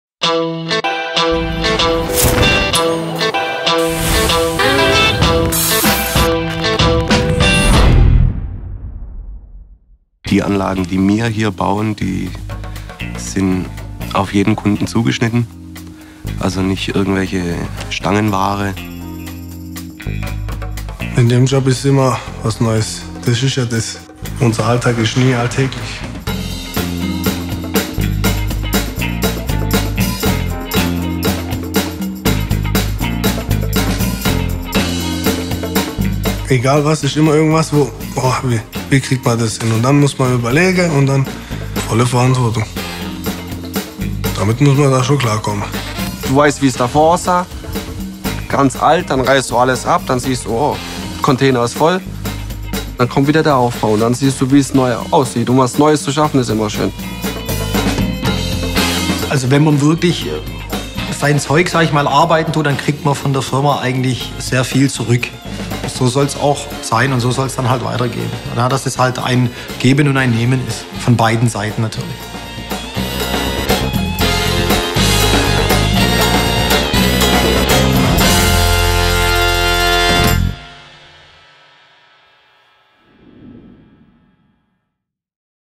Altvater Recruitingfilme - Komm ins A-Team! Folge III: Die Arbeit
blubb.media hat gemeinsam mit der Agentur Bruce B. für die Altvater GmbH aus Magstadt drei Recruitingfilme produziert, die sich von der Masse abheben und so über die sozialen Netzwerke potenzielle Bewerber ansprechen sollen. Dafür haben wir das eingespielte Team des Bäderbetriebes einen Tag lang mit mehreren Kameras begleitet.